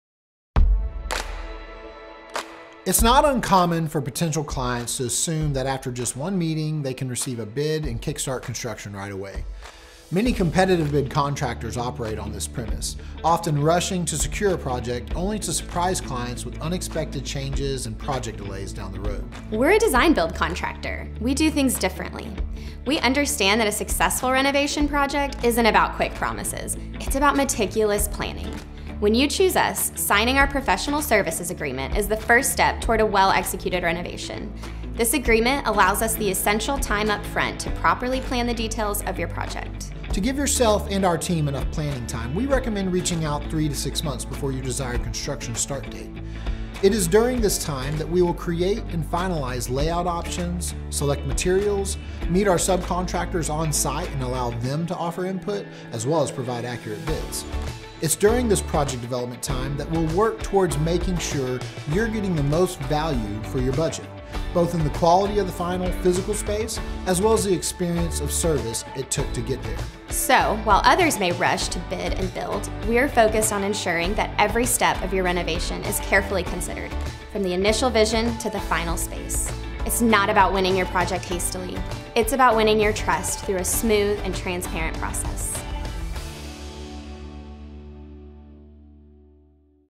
Why does it take so long to get a bid for a remodel?
When you choose us, a design-build contractor, you'll be receiving the benefits of planning BEFORE construction begins. That means materials are selected and ordered, subcontractors have visited the site and been scheduled, fixed cost bids have been received, and transitions and details have been thought through. You'll not only get the final space you're envisioning, but the experience to get there will be much more smooth than if you had to make decisions during construction.

Of course, changes can and do come up during construction, both changes of necessity and requested changes by our clients, and we are prepared and willing to work with these changes. However, the more design and planning that can be done ahead of time, the better the experience and final result!